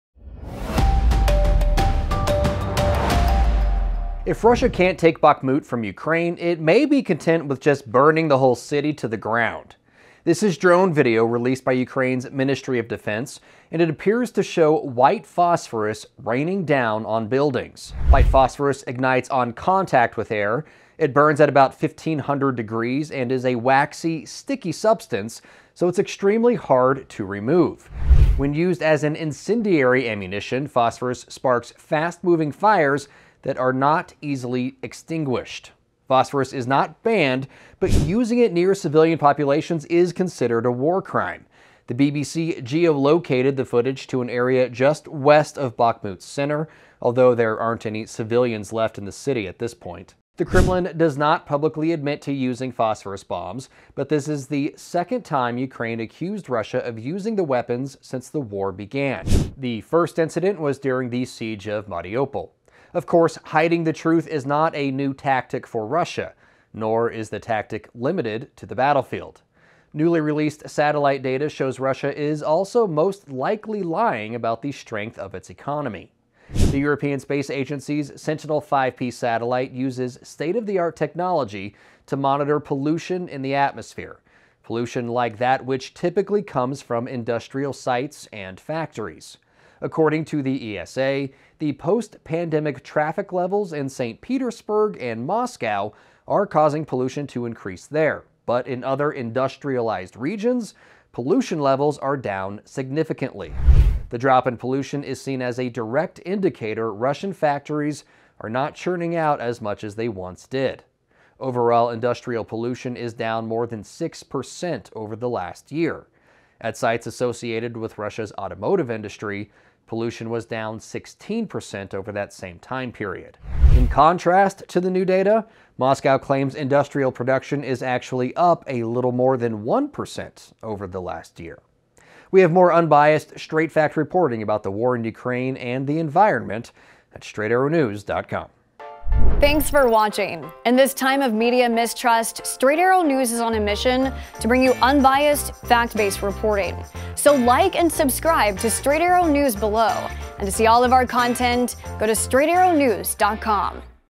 Ukraine says Russia bombed Bakhmut with white phosphorus
If Russia can’t take Bakhmut from Ukraine, it may be content with just burning the whole city to the ground. In drone video released by Ukraine’s Ministry of Defense, images show what appeared to be white phosphorus raining down on the city’s buildings.

White phosphorus ignites on contact with air. It burns at about 1500 degrees, and is a waxy, sticky substance so it’s extremely hard to remove. When used as an incendiary ammunition, phosphorus sparks fast-moving fires that aren’t easily extinguished.

Phosphorus isn’t banned but using it near civilian populations is considered a war crime. The BBC geo-located the footage to an area just west of Bakhmut’s center, although there aren’t any civilians left in the city at this point.